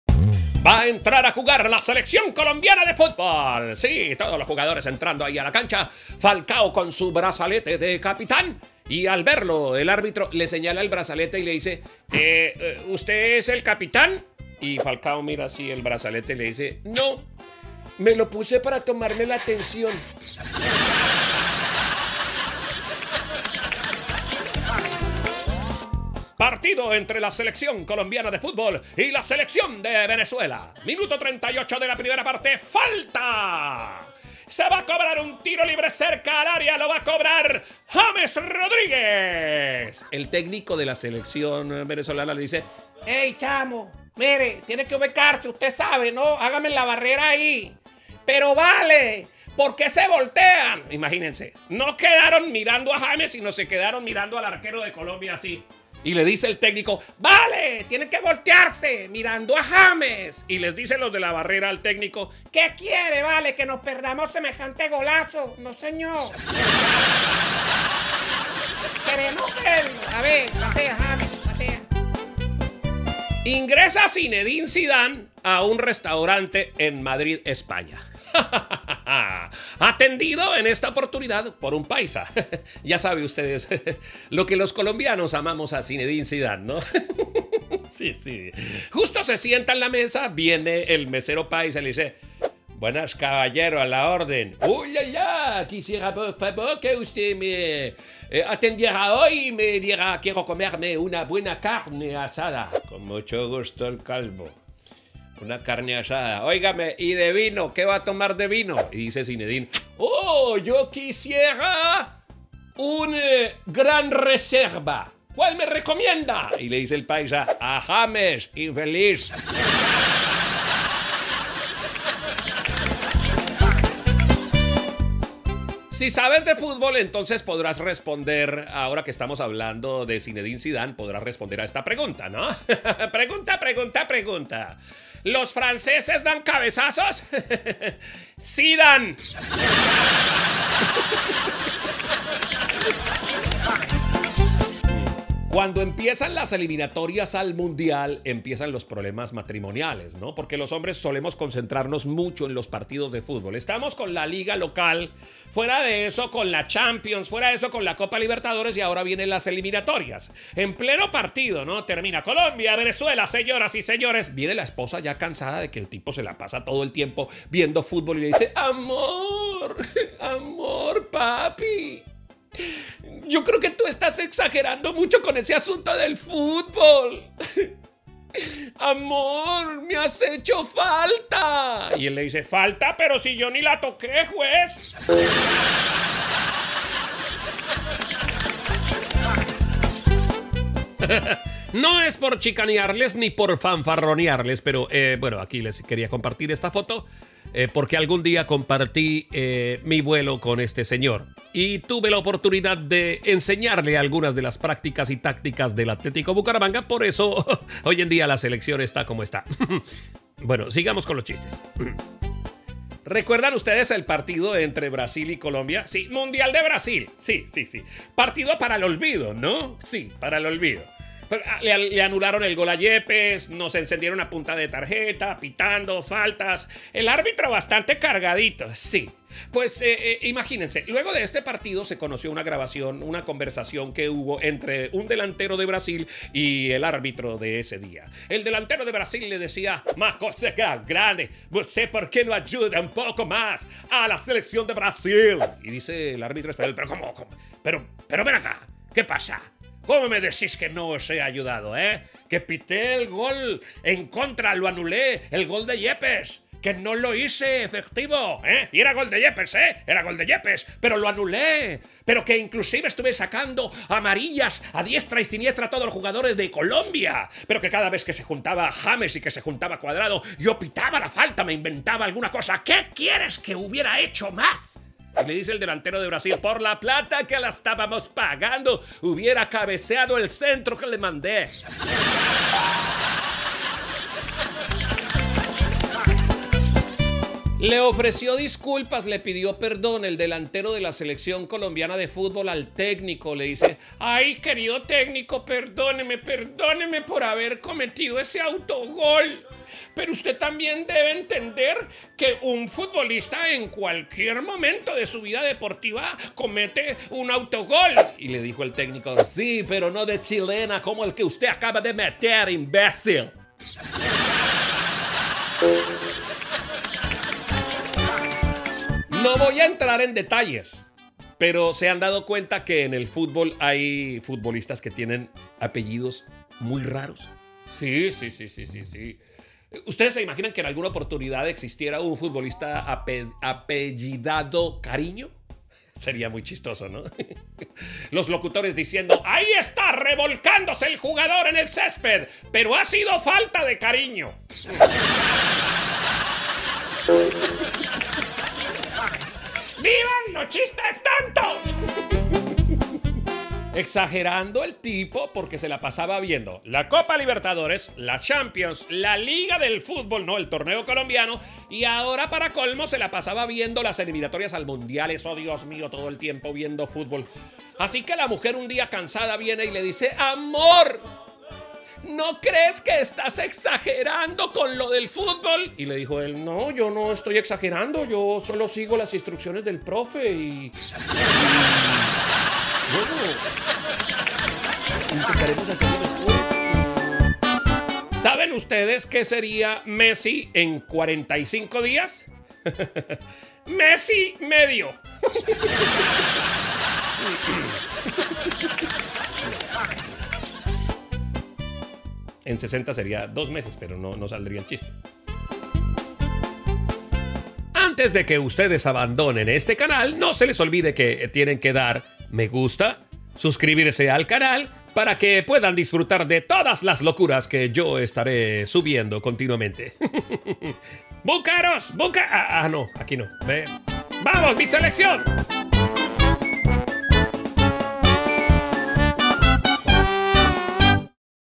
¡CHISTES DE LA SELECCION COLOMBIANA DE FÚTBOL! José Ordóñez 2020
Aquí hay una pequeña selección de chistes de la selección Colombiana de fútbol aprovechando las eliminatorias al mundial de Qatar 2022. Disfrútalos.